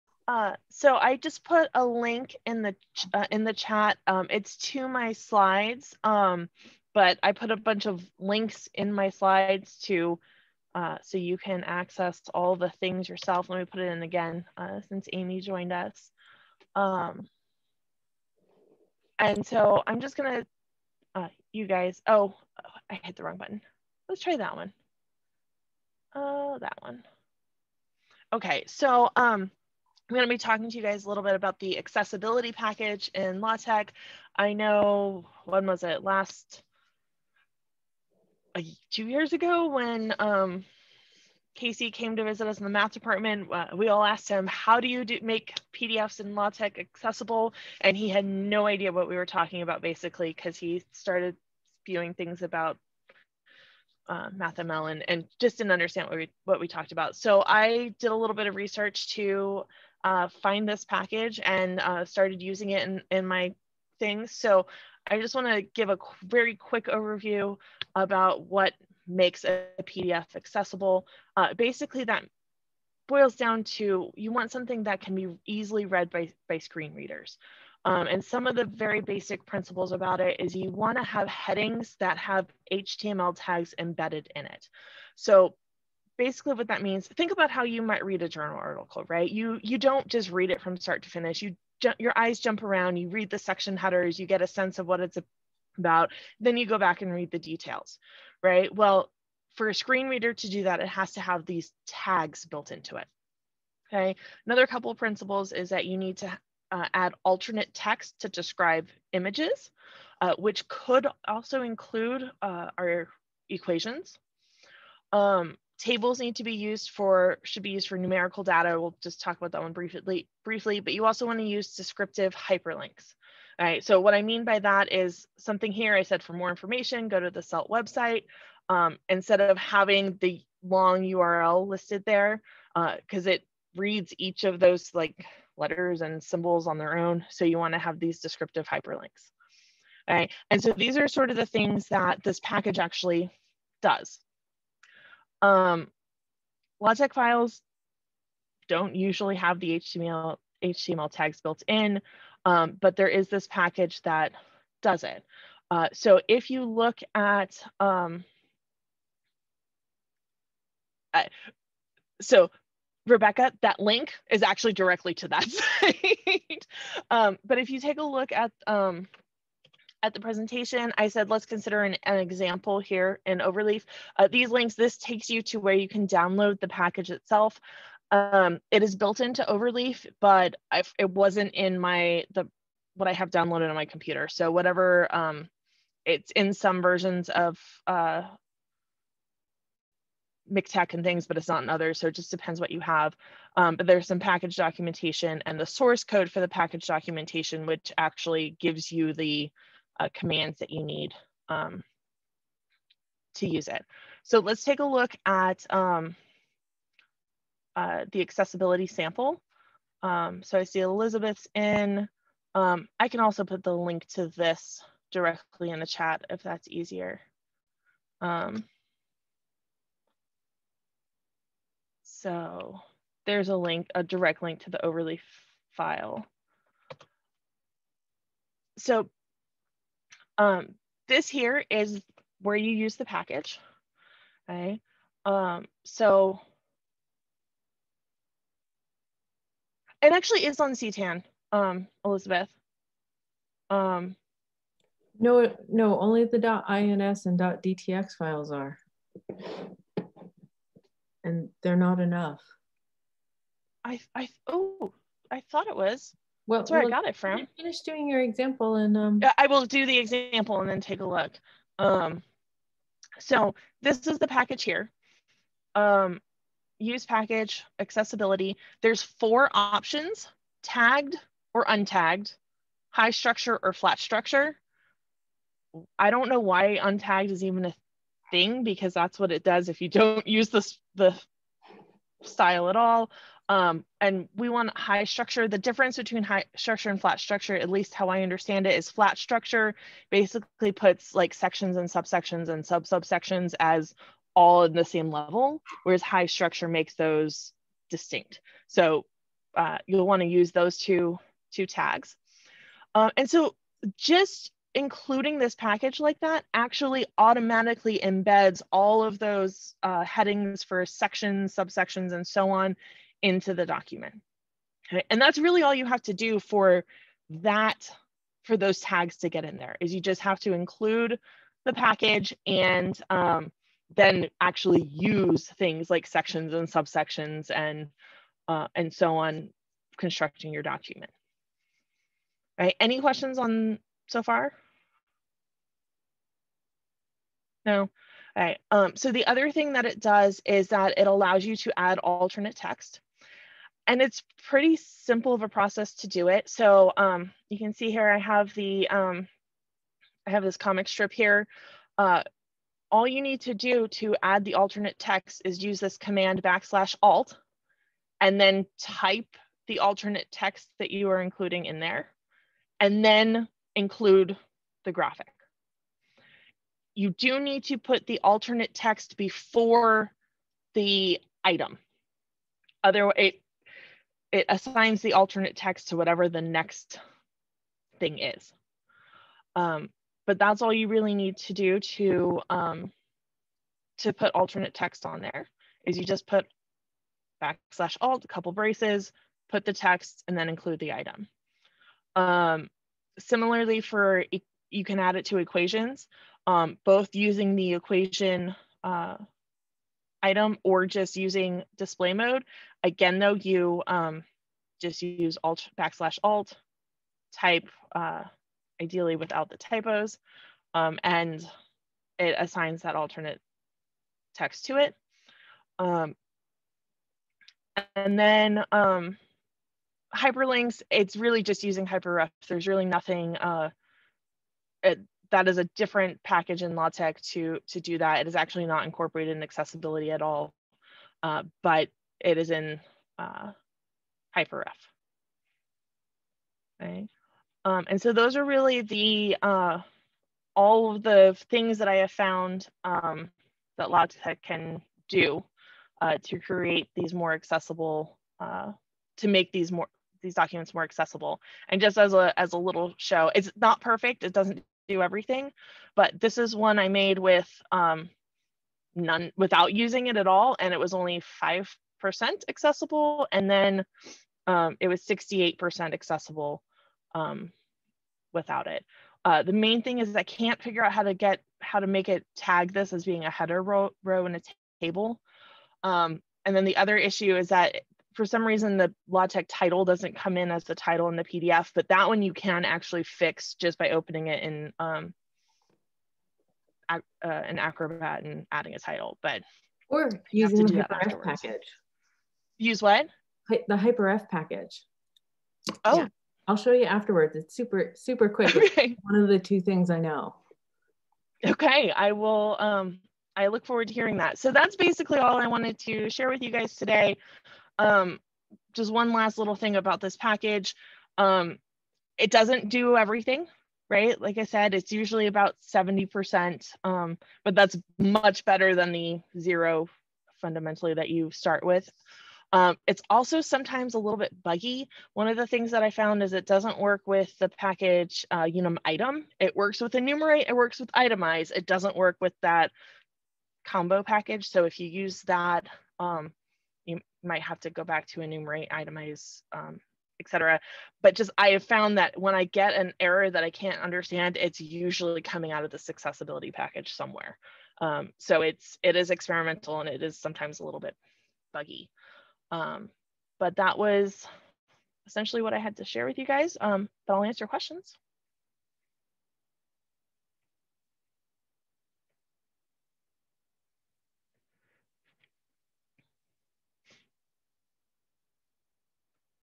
98. The Accessibility Package in LaTeX
It is a challenge to create documents in LaTeX that meet the accessibility principles. One package that can improve the accessibility of your LaTeX documents is accessibility.sty. In this session, I will help you download the style file, and demonstrate the commands provided by the package.